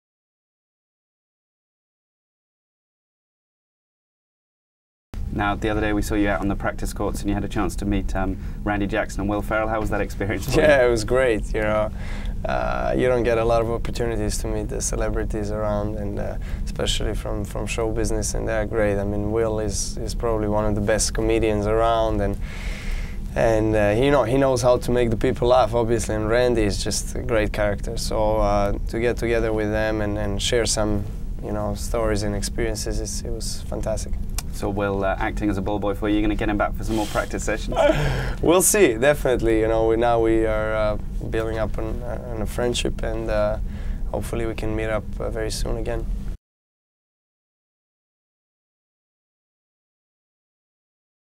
Will Ferrell ball boys for Novak Djokovic
Will Ferrell became a ball boy for Novak Djokovic during a practice session at Indian Wells in 2011.